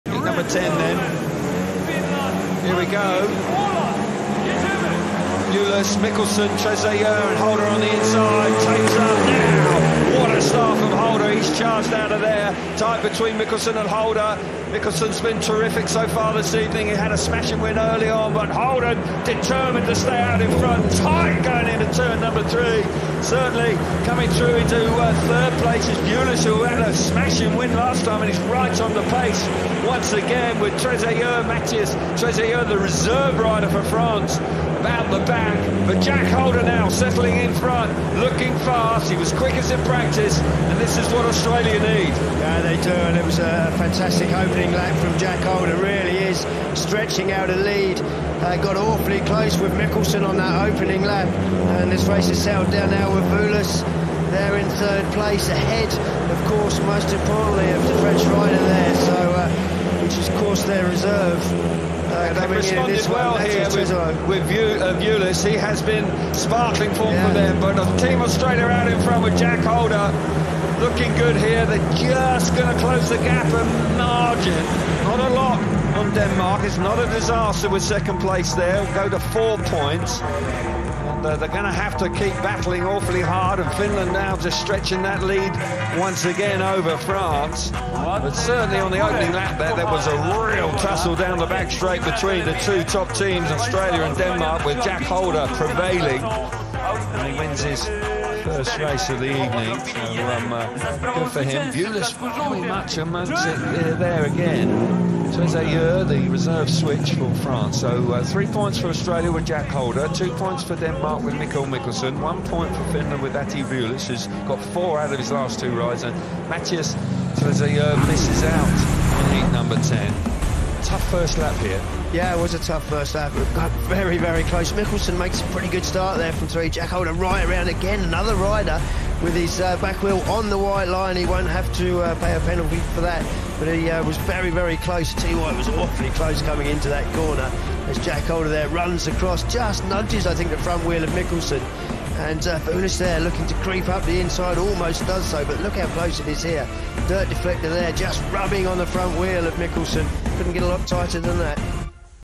How much speed did Jack Holder have?! 😱 Heat 10 SF2 SWC | FIM Speedway Grand Prix
That was some ride from Jack Holder 👏

Just as the Danes were starting to look ahead to the Final, Jack Holder reminded them how much of a threat Team Australia are 🔥